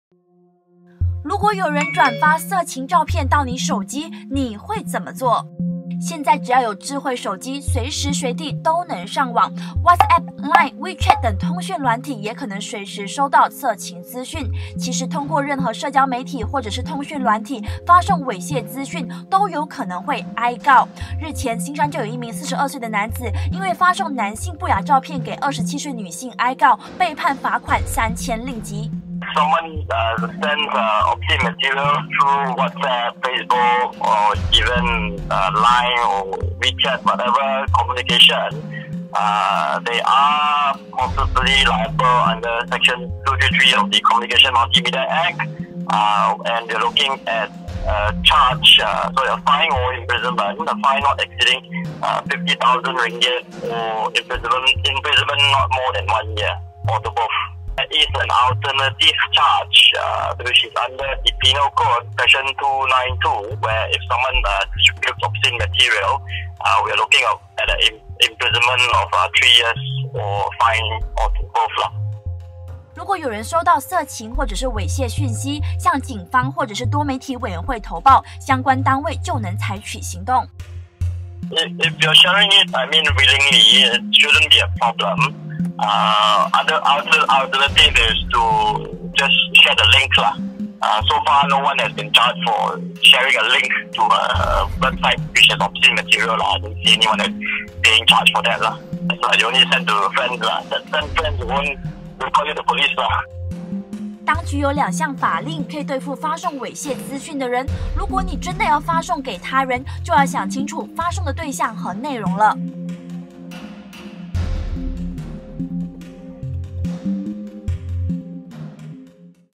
生活：手機收到色情簡訊怎麼辦？ 20141212
如果有人轉發色情照片到你手機，可以怎麼辦？傳送色情資訊，其實有可能被兩項法令對付，刑罰又是什麼呢？